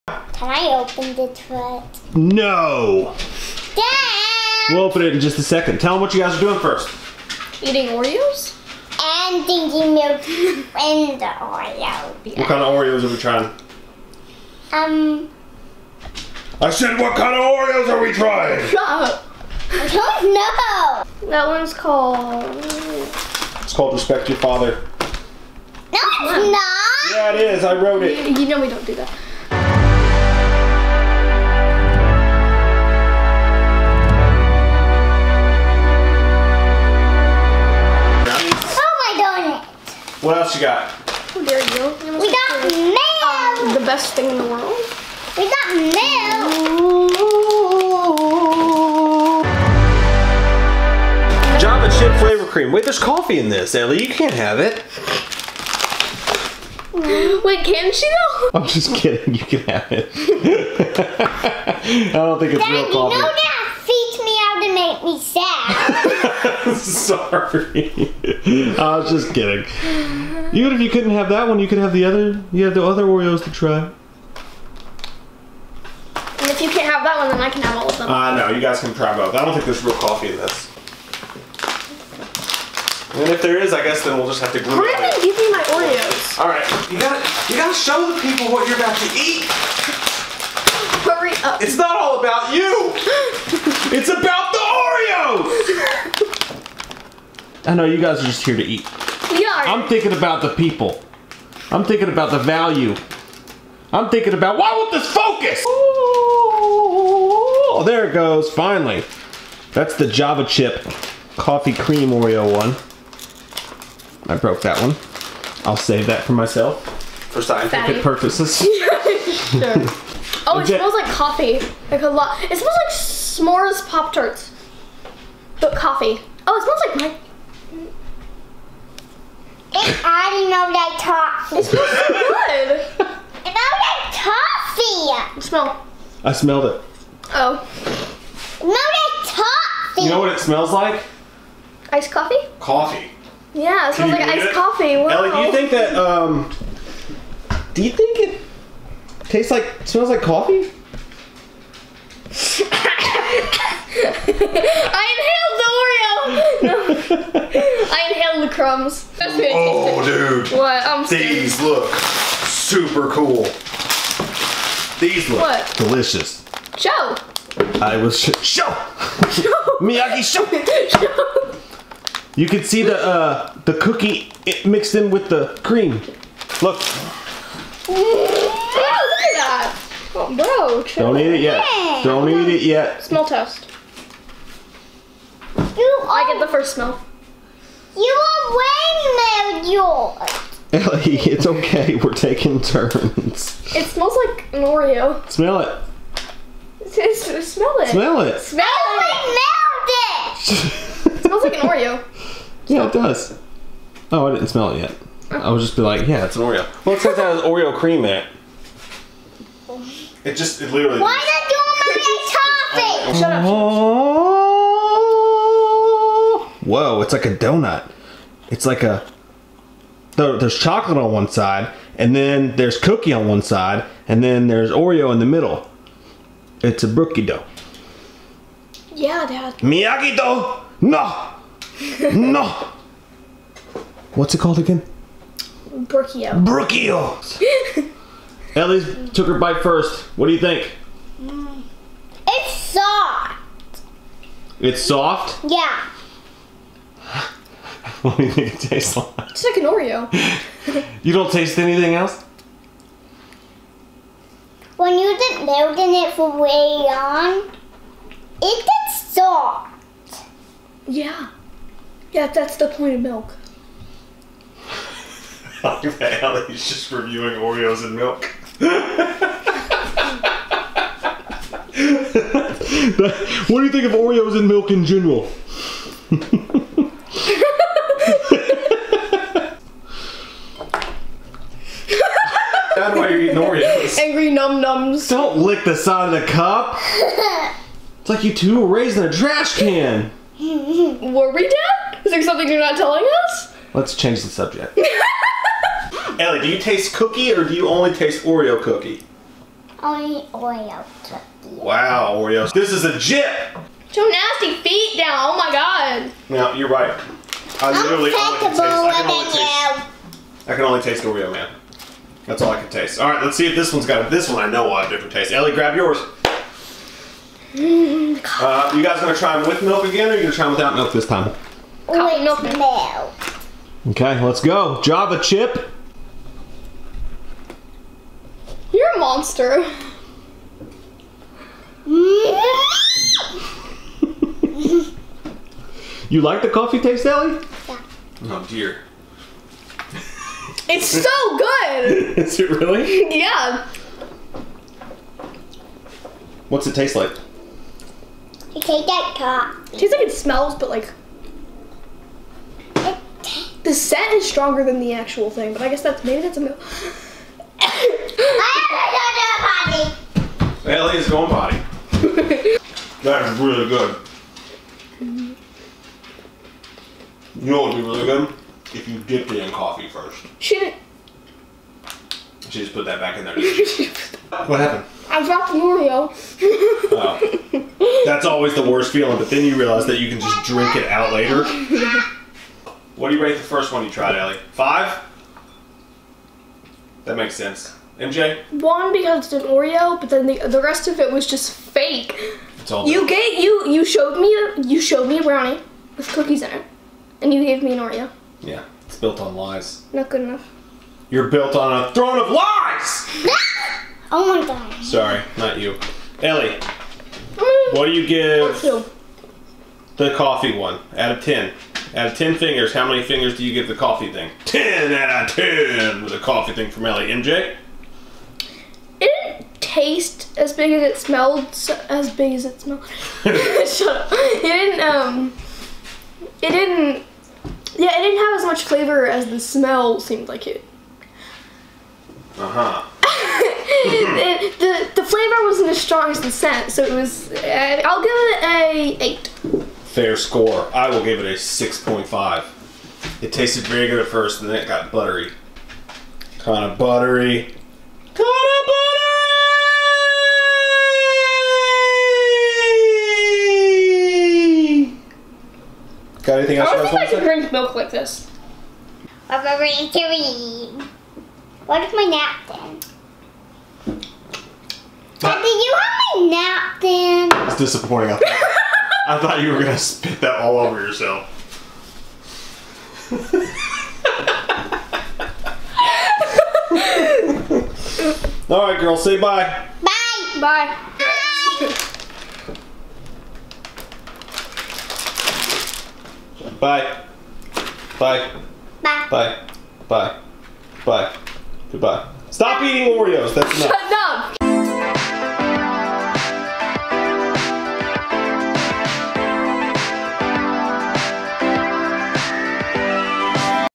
Oreo Taste Tests! Java Chip + Brookie Dough
On today's video the girls try two of Oreo's newest creations. The Oreo Java Chip Coffee flavored oreo and the Brookie Dough Cookie Dough Oreo!